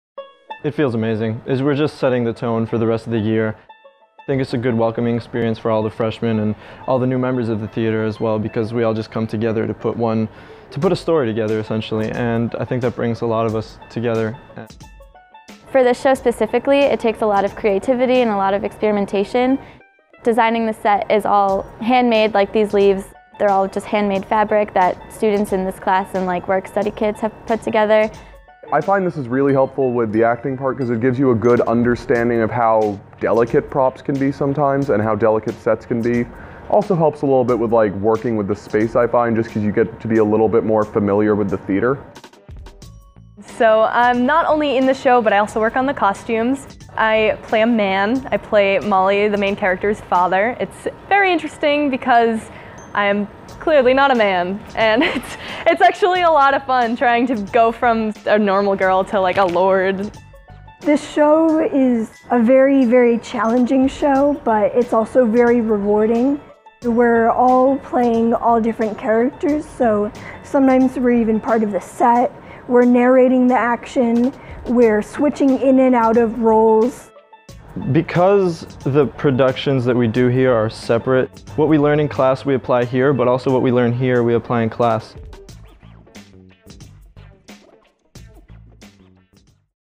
Peter & The Starcatcher
Students involved in our Theatre program experience all aspects of putting on a show, from start to finish. By exposing them to a variety of job roles in the theatre, they can focus on where their interests are. Cast members enrolled in Theatre Practicum put the finishing touches on costumes and sets in preparation for The Barn's first show of the season!